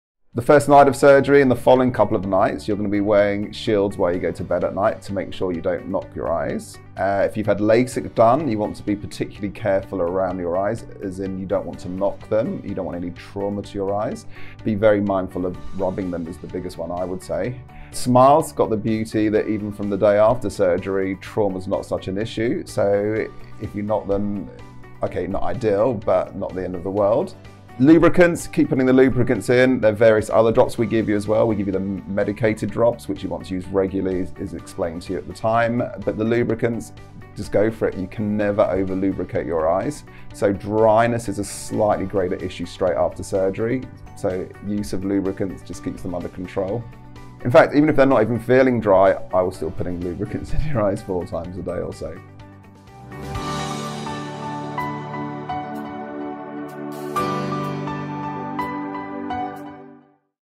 How should I take care of my eyes the first week after laser eye surgery?
In this video, Dr David Cropman explains how to ensure that you take optimum care of your eyes following your surgery. He describes the ideal care following particular procedures, such as LASIK and SMILE, and explains the benefit of using the lubricant provided to avoid dry eye. Watch to find out more...